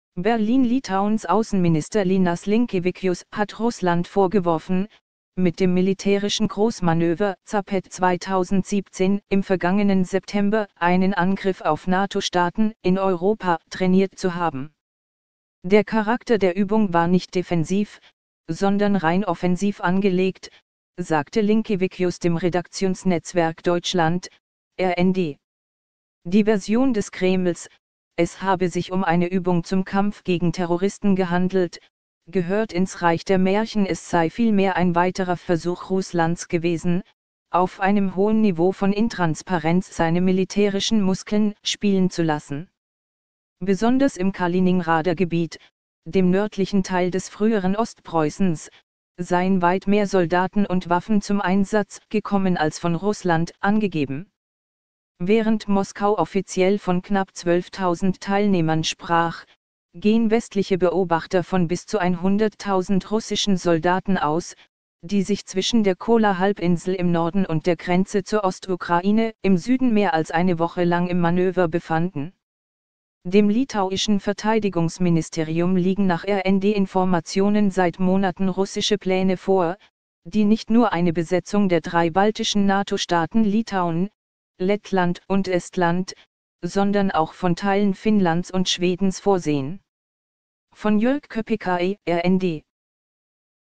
Militärische Muskelspiele – Litauen sieht konkrete Bedrohung durch Russland
Militärische Muskelspiele –
Litauen sieht konkrete Bedrohung durch Russland